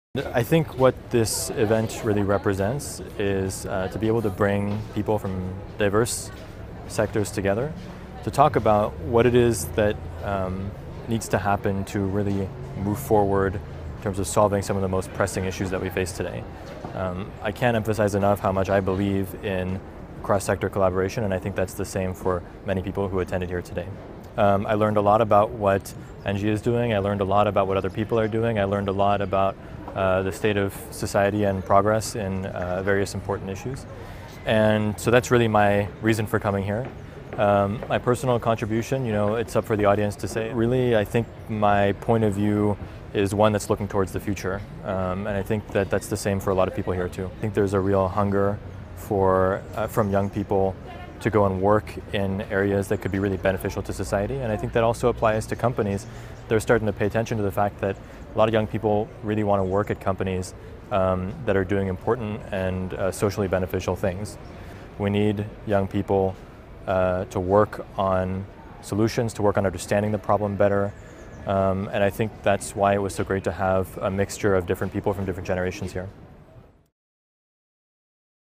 The good day : Parker Liautaud (ENG)
Interview with Parker LIAUTAUD, PhD candidate, Departement of Earth and Planetary Sciences, Harvard University, at The good day, June 20, 2019 in Paris.